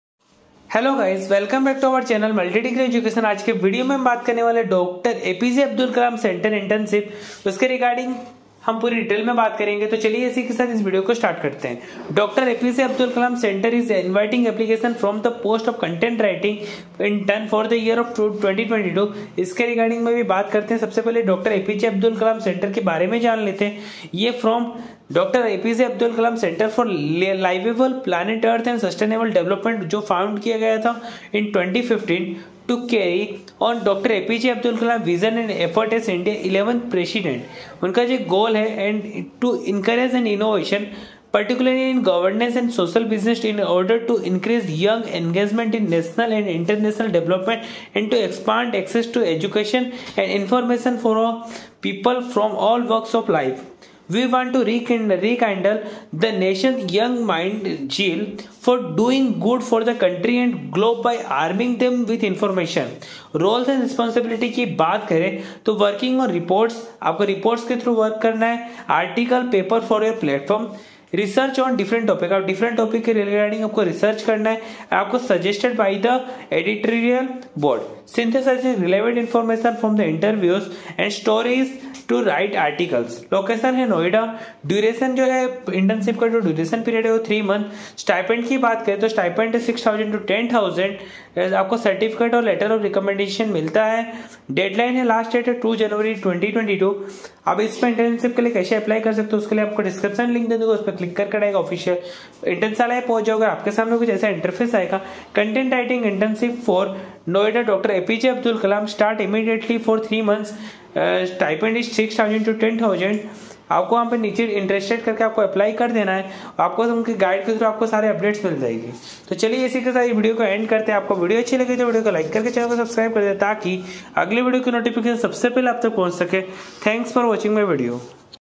Dr APJ Abdul Kalam Center Hiring Interns | APJ Abdul Kalam Center hire intern on best stipend | MDE
Is video me ham aapko Dr APJ Abdul Kalam Center abhi fresher interns hire kar rahe hai for internship Iske regarding full details dene waale hai.